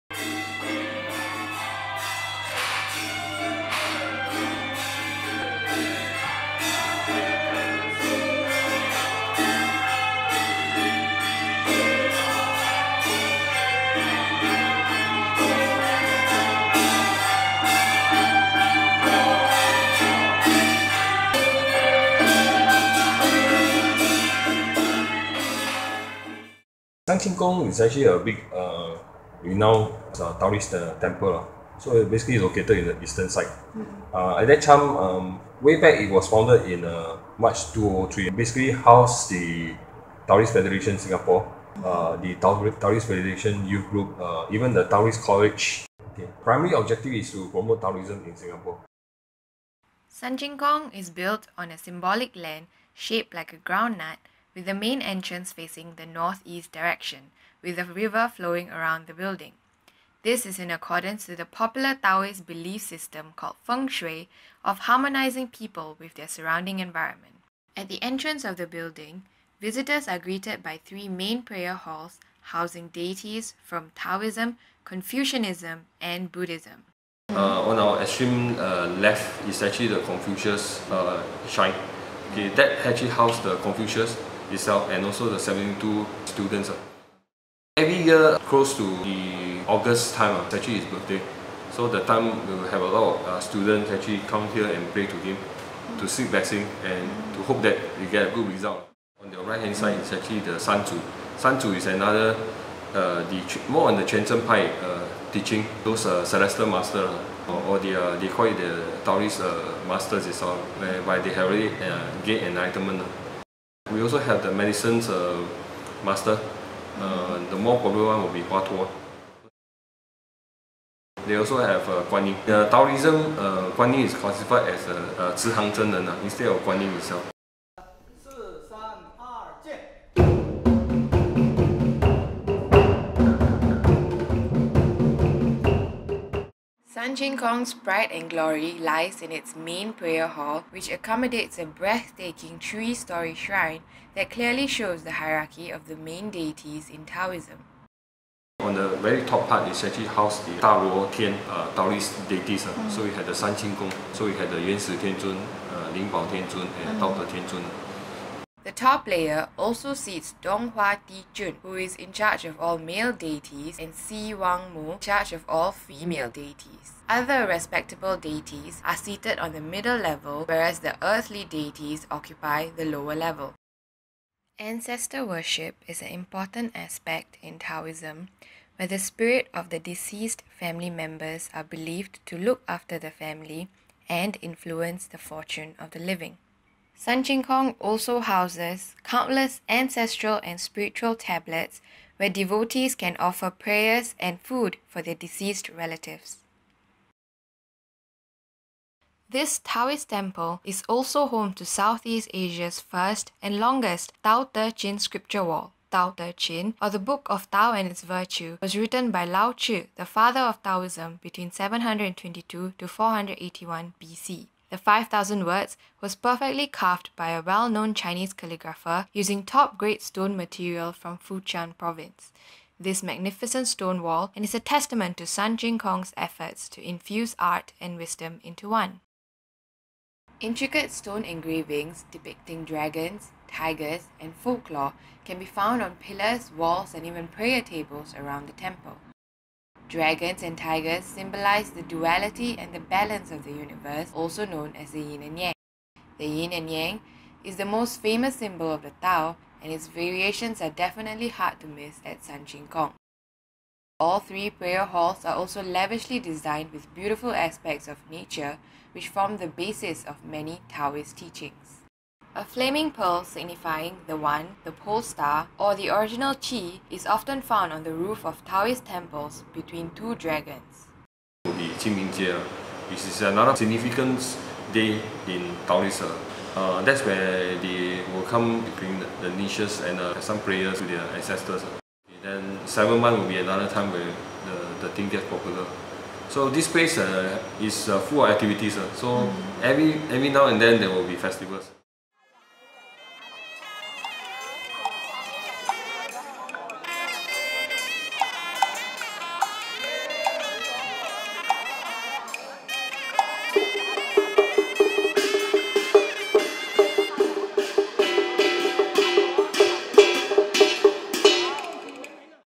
DD8008: San Qing Gong Taoist Temple
Explore the beauty of Taoist practices and art at San Qing Gong Taoist Temple.

DD8008: Faith and Art

By: Naveeda Hussain

2. xserra, Pekin-Univ-Orchestra-1.wav